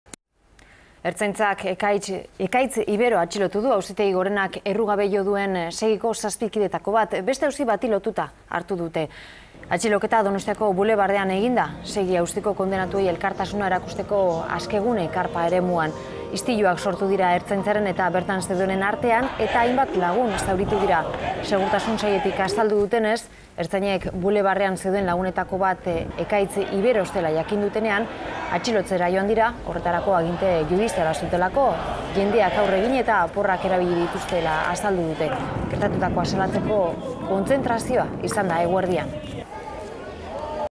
Incidents au cours de l'arrestation d'Ekaitz de Ibero à Donostia
L'Ertzaintza a arrêté Ekaitz de Ibero, membre présumé de Segi, à Donostia. Des heurts opposant des militants et la police ont fait plusieurs blessés.